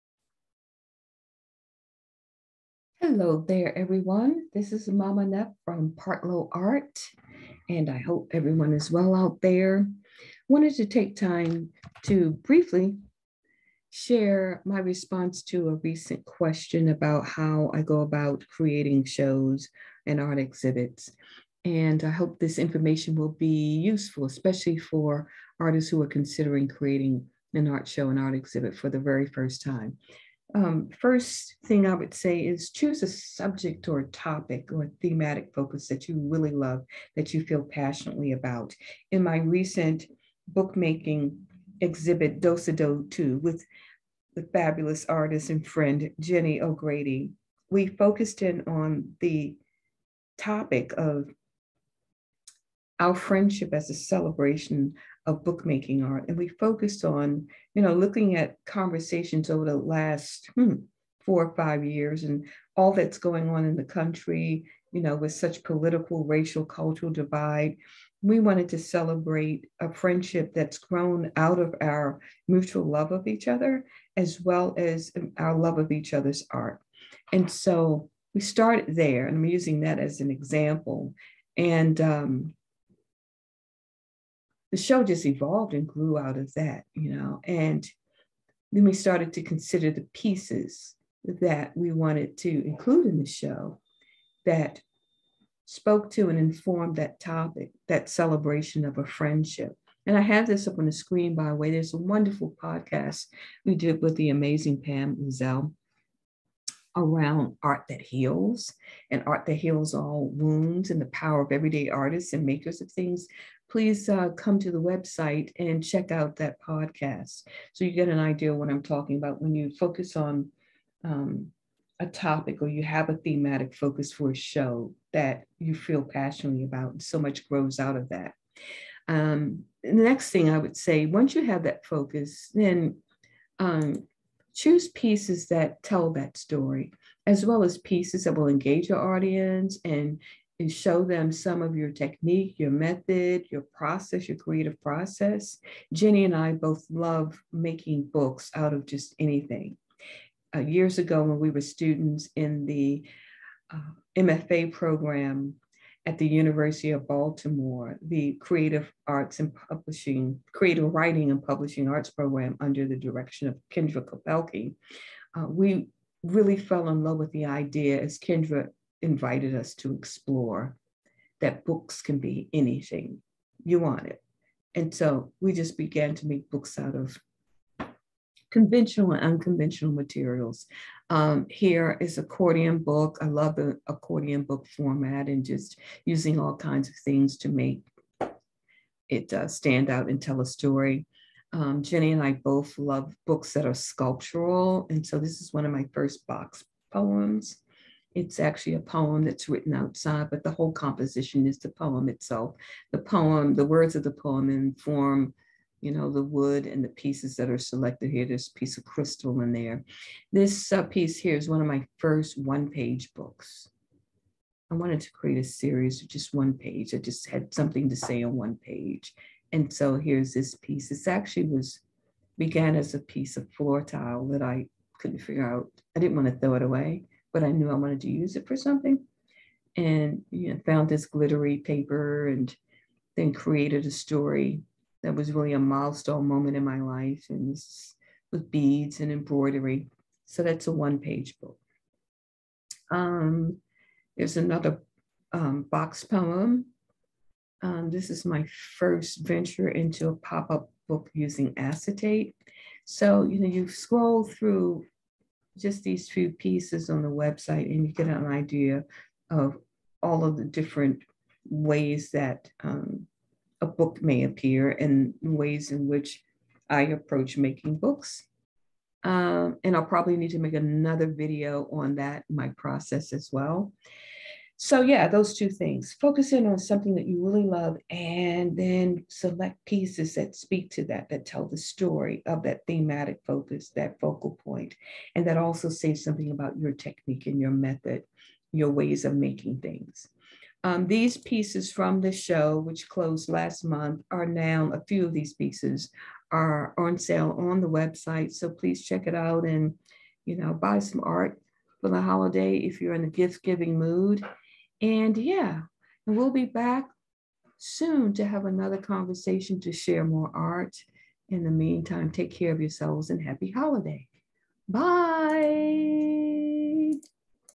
2 Simple Tips for Creating an Art Show
Mama Nef of Partlow Art shares two simple tips for creating a cohesive art show that engages an audience and showcases your unique artmaking techniques. Check out her work at Partlowart.com